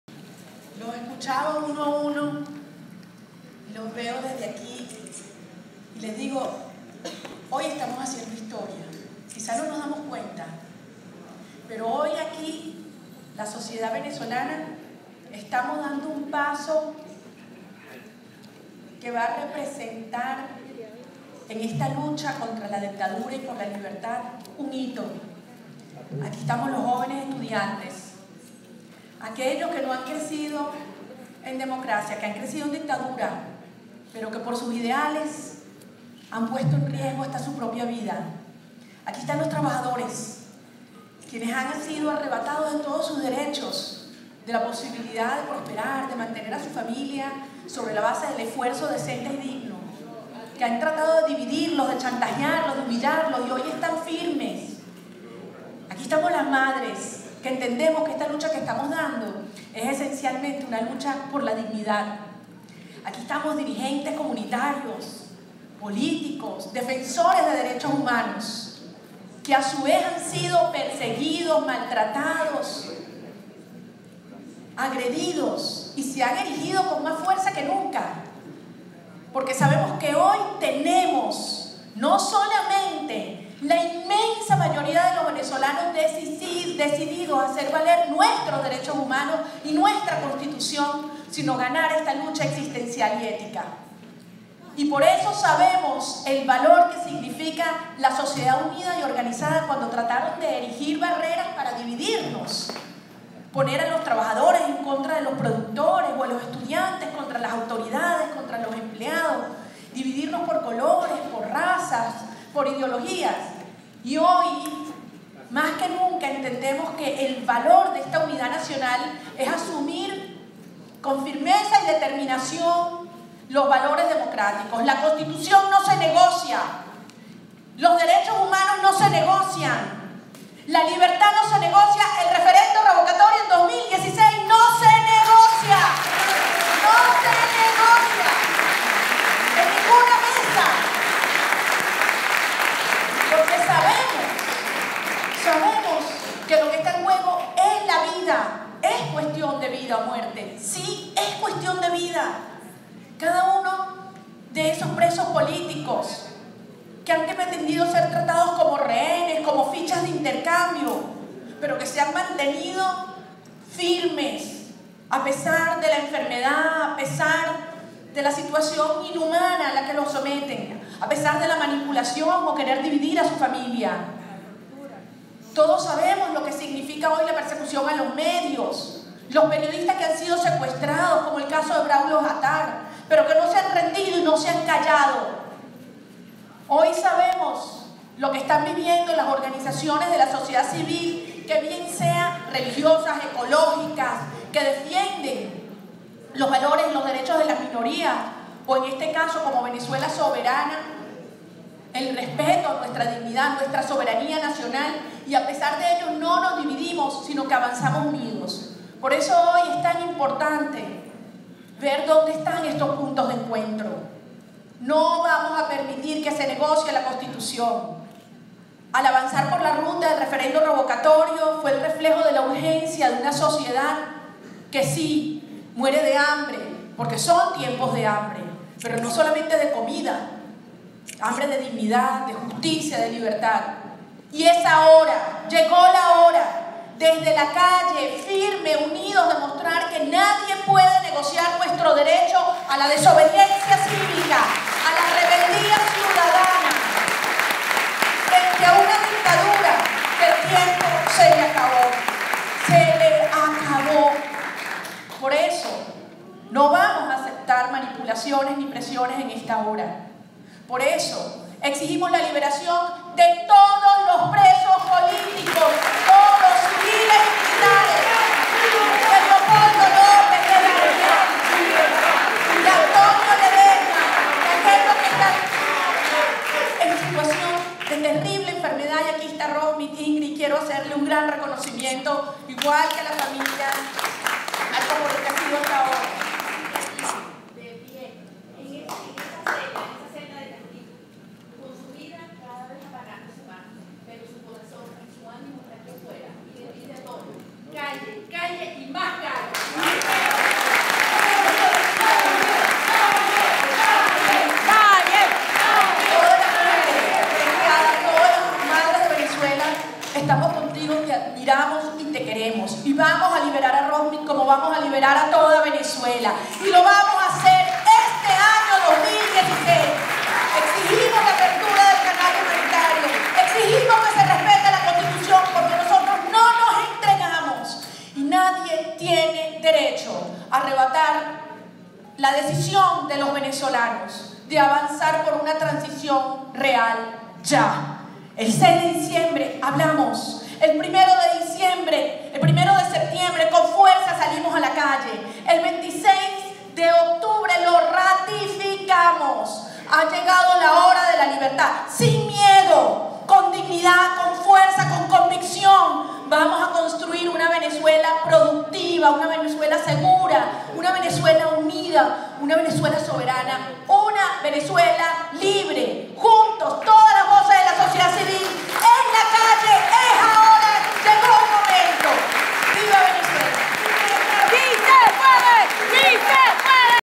María Corina Machado: Ha llegado la hora de la unión social para derrotar al régimen este año
Declaraciones de la Coordinadora nacional de Vente Venezuela durante el foro "Compromiso de los actores sociales por Venezuela".